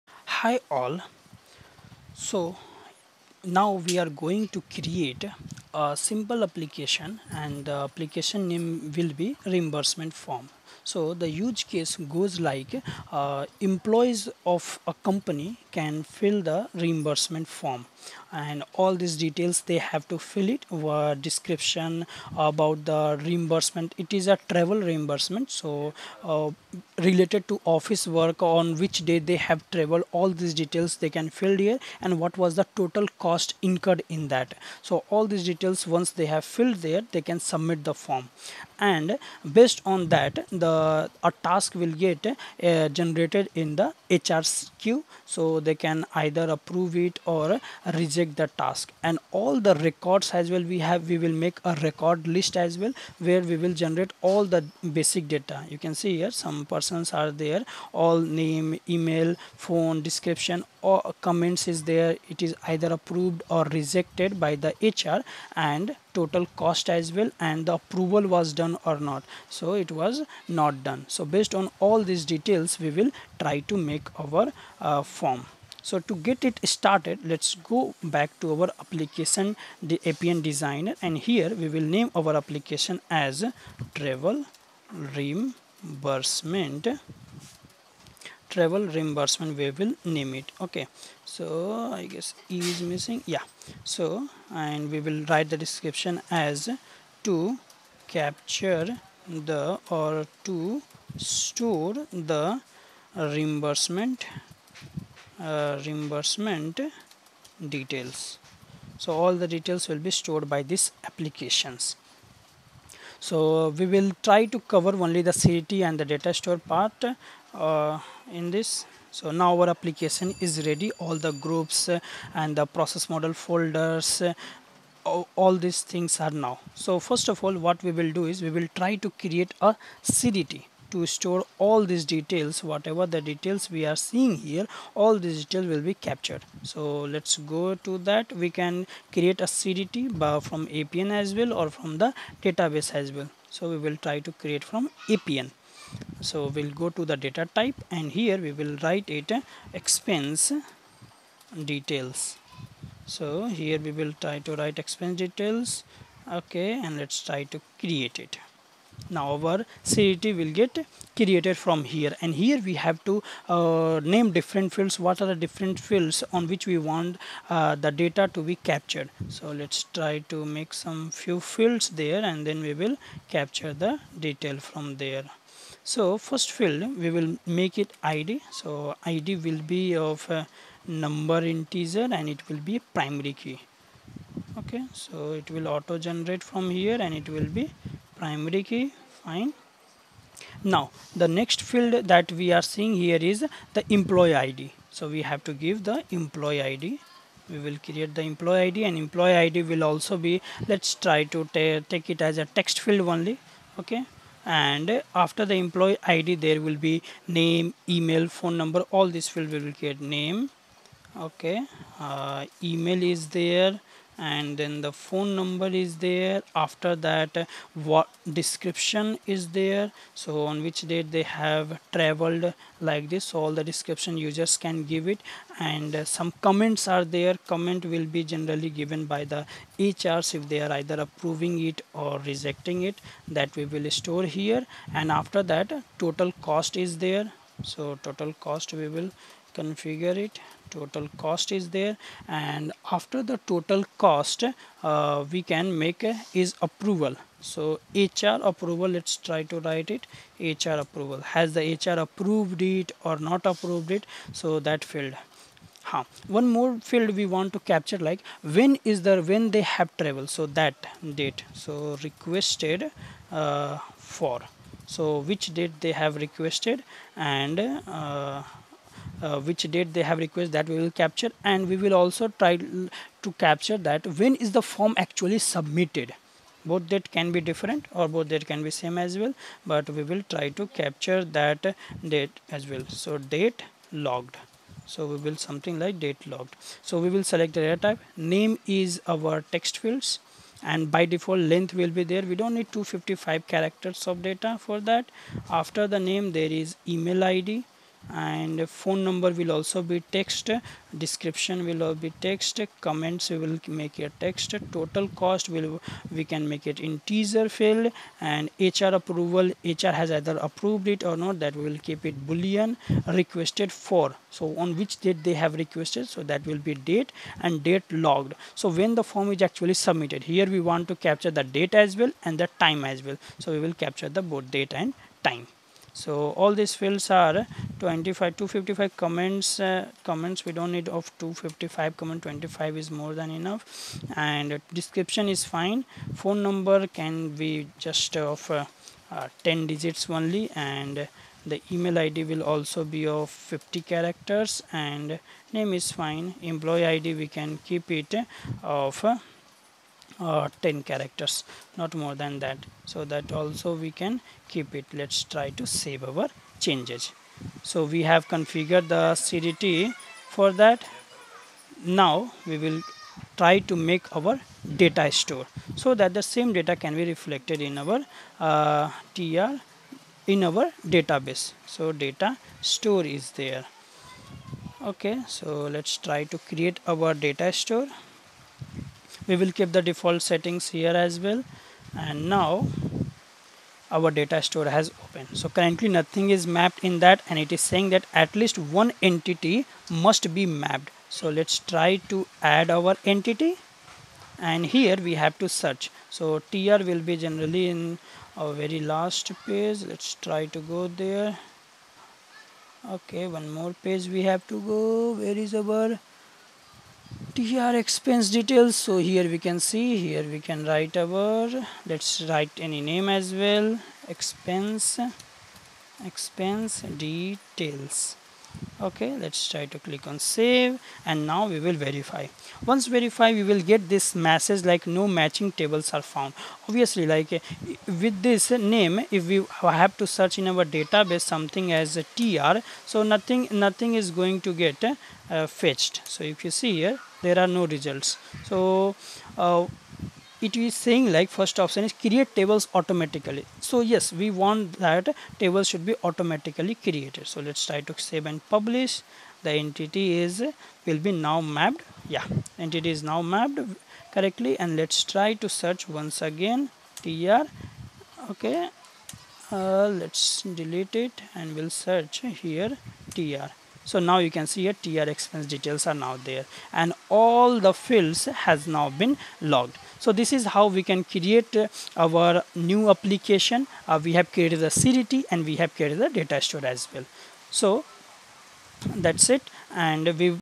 Travel Reimbursement App | Appian POC | Part 1 | Introduction to App, CDT and Data Store
Topics Covered -
1. Introduction to Application "Travel Reimbursement"
2. Create a CDT in Appian
3. Create a Data Store

For more contents and updates
You can join my Telegram channel

appian tutorial for beginners
appian tutorial